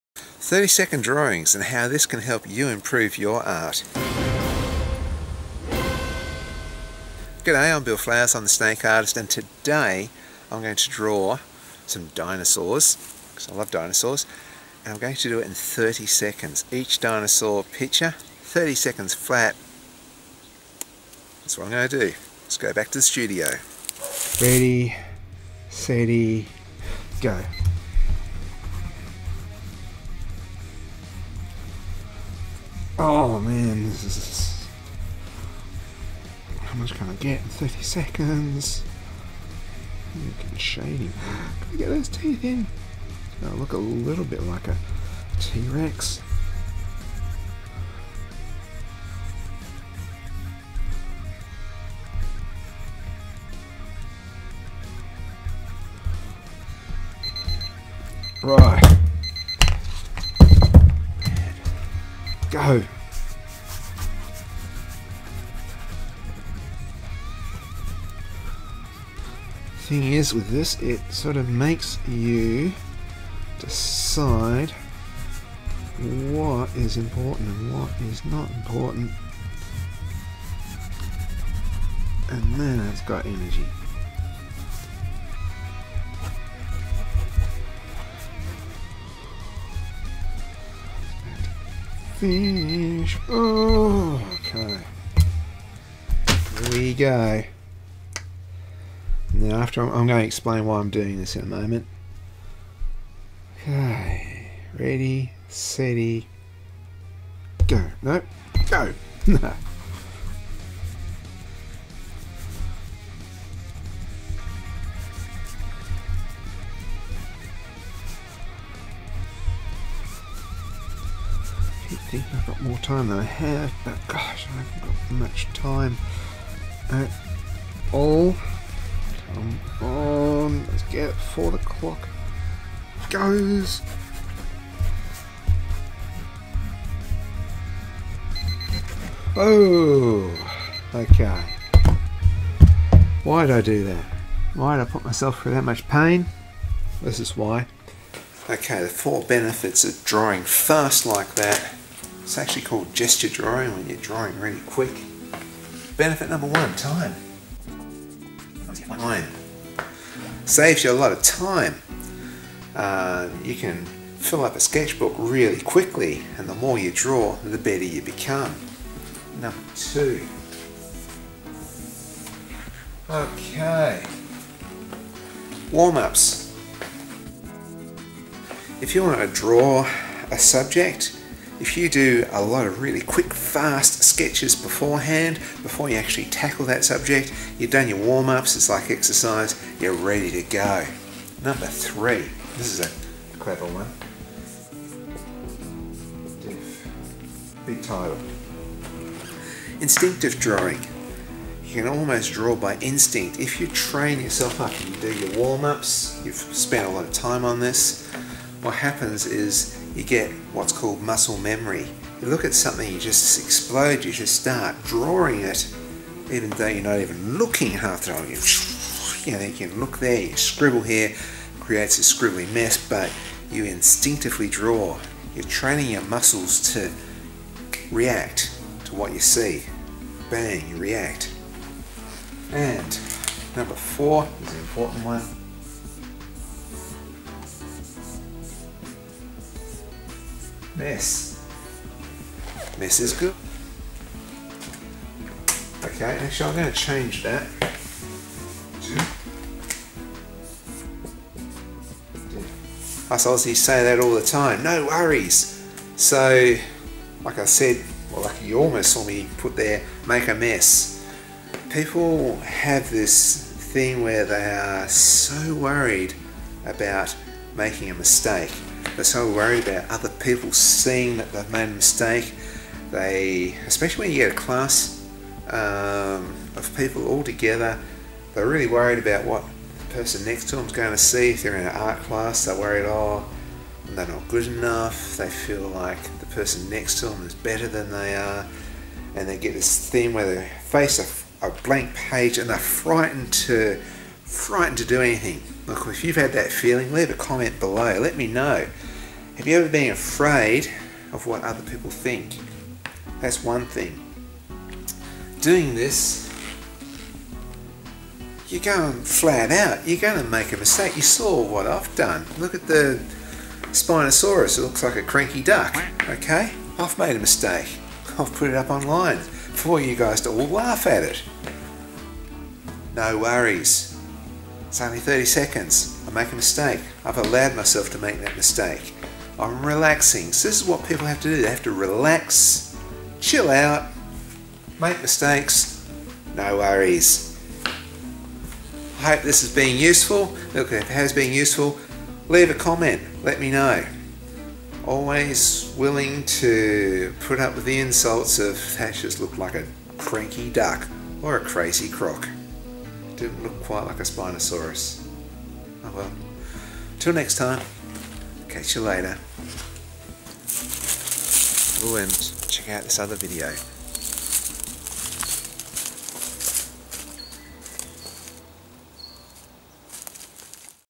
30 SECOND DRAW CHALLENGE & 4 REASONS YOU SHOULD TOO
In this video I draw dinosaurs for 30 seconds. A fun challenge, but why do it. I outline 4 reasons it will improve an artist's skill and take them to the next level. This is a good video for those who feel they are not good enough or are struggling to improve their art.